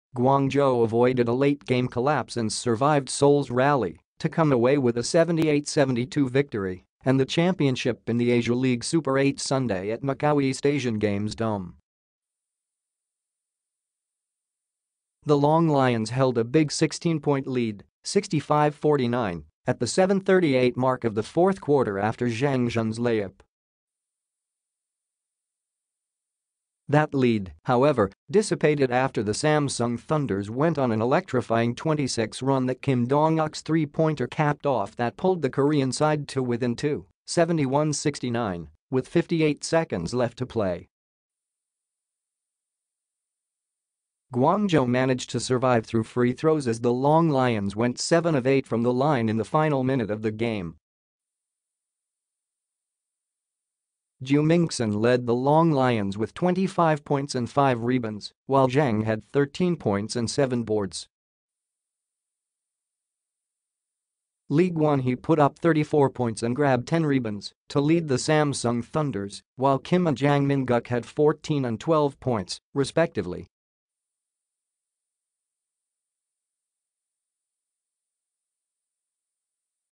Guangzhou holds off Seoul for Asia League title
Guangzhou avoided a late-game collapse and survived Seoul’s rally to come away with a 78-72 victory and the championship in the Asia League Super 8 Sunday at Macau East Asian Games Dome.

The Long Lions held a big 16-point lead, 65-49, at the 7:38 mark of the fourth quarter after Zheng Zhun’s layup.

That lead, however, dissipated after the Samsung Thunders went on an electrifying 20-6 run that Kim Dong Uk’s three-pointer capped off that pulled the Korean side to within two, 71-69, with 58 seconds left to play.

Guangzhou managed to survive through free throws as the Long Lions went 7-of-8 from the line in the final minute of the game.

Ju Mingxin led the Long Lions with 25 points and five rebounds while Zheng had 13 points and seven boards.

Lee Gwan Hee put up 34 points and grabbed 10 rebounds to lead the Samsung Thunders while Kim and Jang Min Guk had 14 and 12 points, respectively.